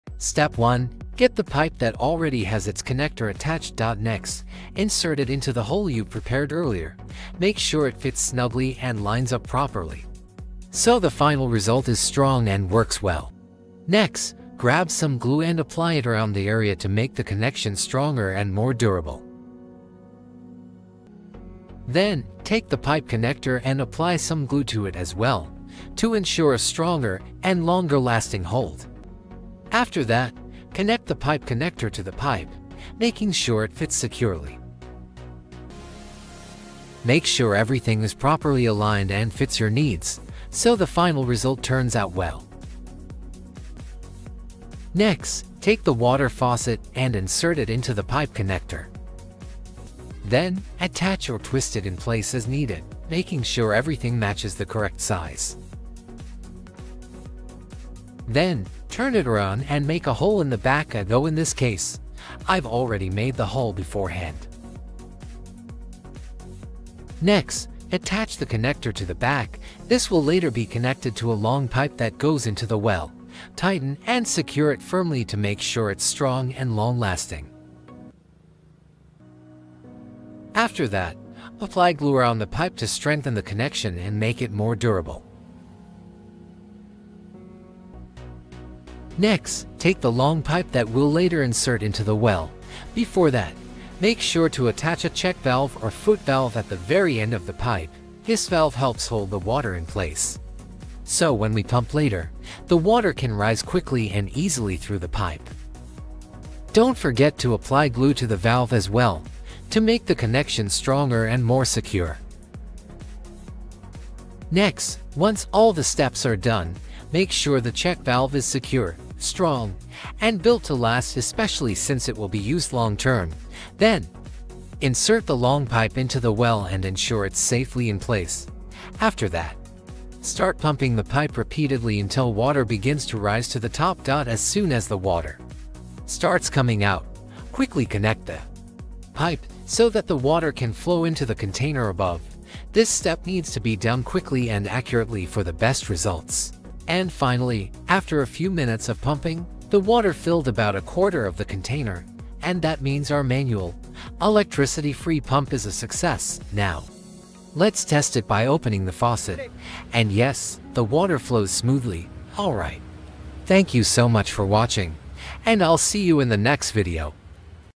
Water pump with no electricity - free for life! most people don’t know this trick!
Discover this amazing DIY water pump that works without electricity and can last a lifetime! Most people have no idea this simple trick exists. Perfect for off-grid living, gardening, survival, or emergencies—cheap, effective, and easy to make!